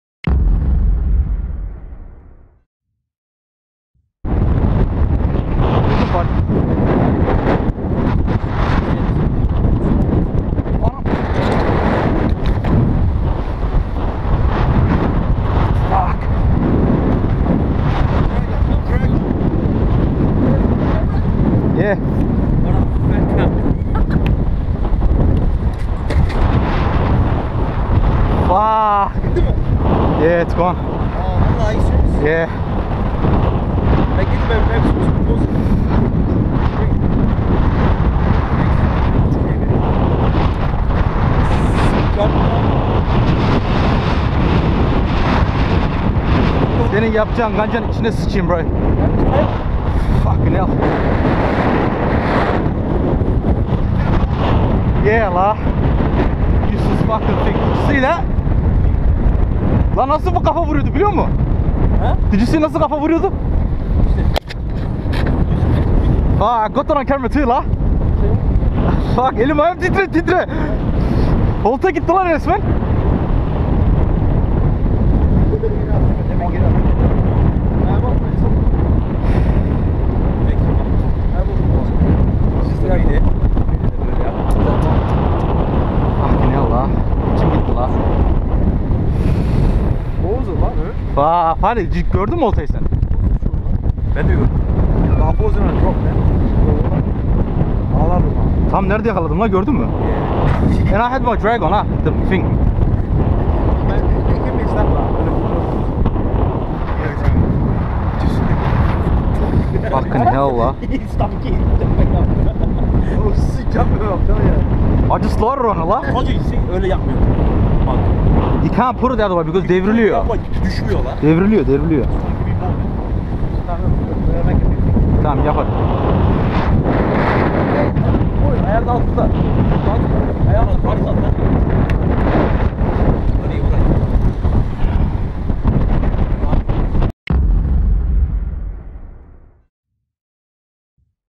15kg Mulloway fish got away
Some said was a Mulloway some said GT. Far as I know was something massive. Thanks to this there will be a story to tell and and go with the flow on every fishing story yet to come.